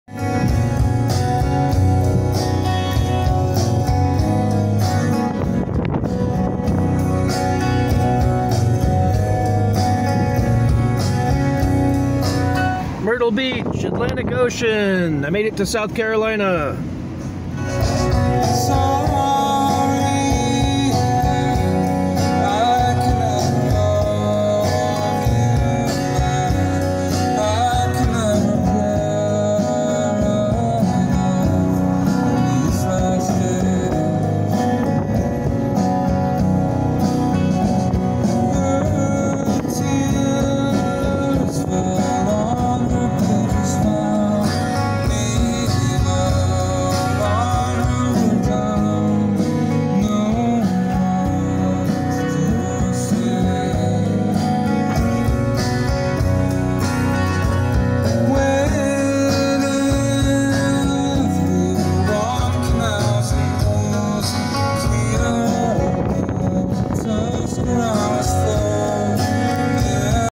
After 2 years away, BACK to ATLANTIC OCEAN, at MYRTLE BEACH. TRAVEL.Nick123.com
After moving away from the Atlantic coast 2 years ago, I moved to the Pacific Northwest - Seattle, Portland and McCall Idaho.  **Now today I return once again to the ATLANTIC SEASHORE .. At Myrtle Beach, South Carolina.  1st time ever here.

- Nick Smyka